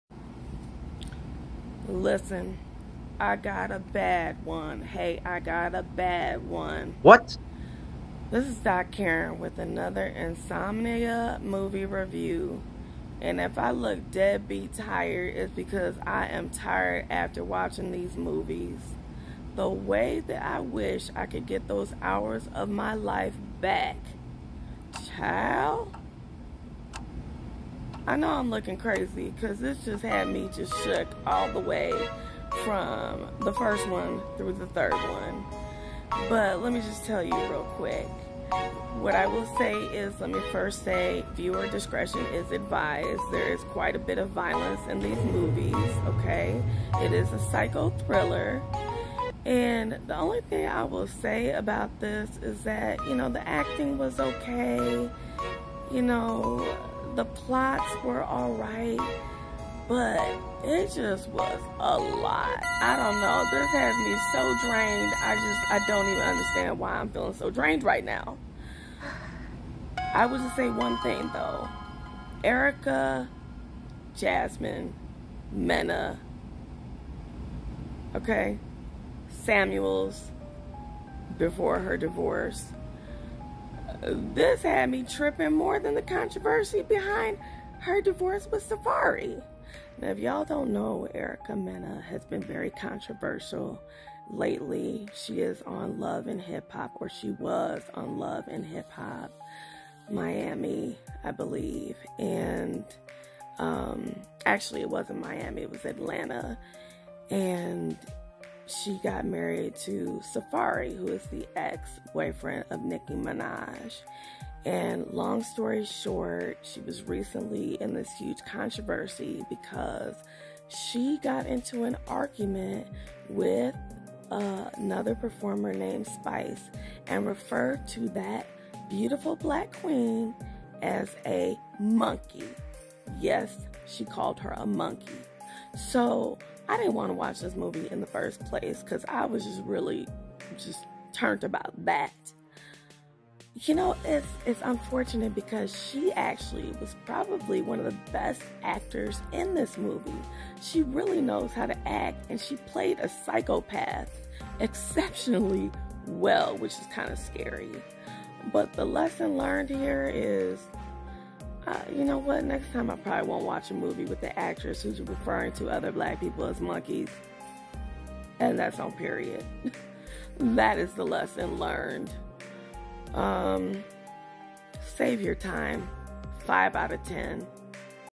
Insomnia Movie Review: The StepMother 1, 2, and 3 featuring Erica Mena. Fair use of images. 5/10
I wish I could rewind time. This woman just running around killing folks for no good reason! The End. Ugggggh "The StepMother". Waste of my time, especially because I'm boycotting this actress from now on. At least until she takes accountability! Fair use of images for educational and entertainment purposes. tubimoviereviewer insomniamoviereviewwithdockaren EricaMena safarisamuels Safari loveandhiphopatlanta bluemonkey loveandhiphopnewyork racism spiceloveandhiphopatlanta colorism loveandhiphop omarion foryoupage badmovies TheStepMotheronTubi #